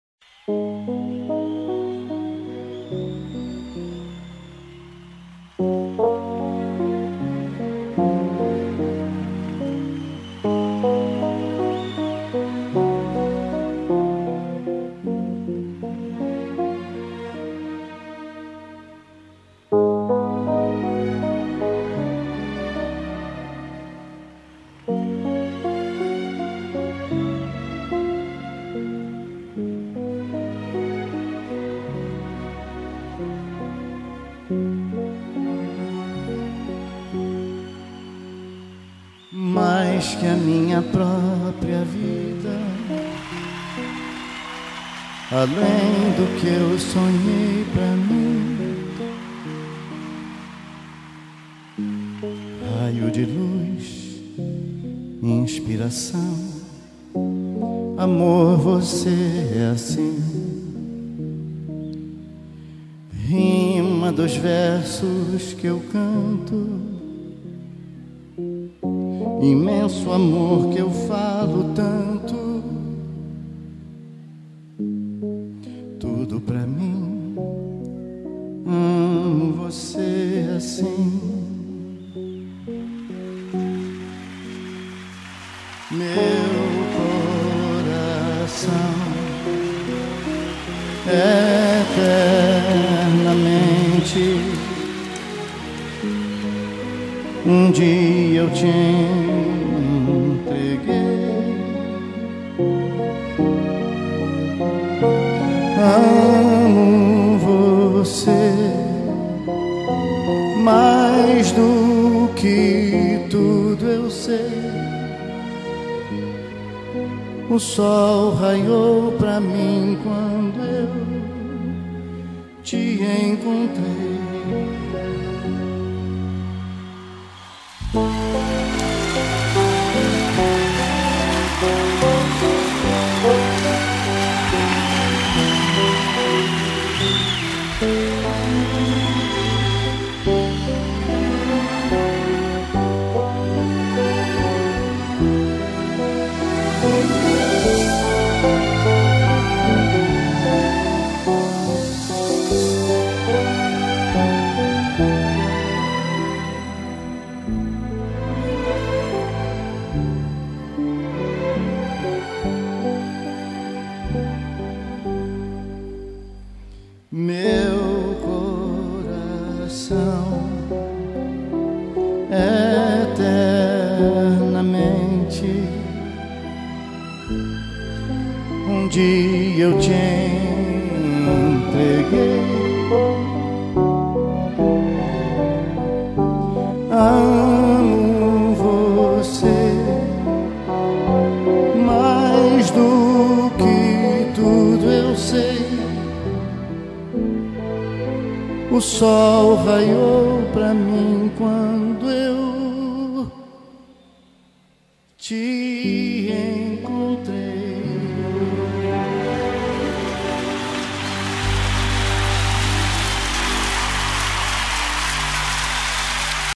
Maria Rita
M ais que a minha própria vida
A lém do que eu sonhei pra mim
R aio de luz
I nspiração
A mor você é assim
R ima dos versos que eu canto
I menso amor que eu falo tanto
T udo pra mim
A mo você assim
M eu coração
E ternamente
U m dia eu te entreguei
A mo você
M ais do que tudo eu sei
O sol
R aiou pra mim quando eu te encontrei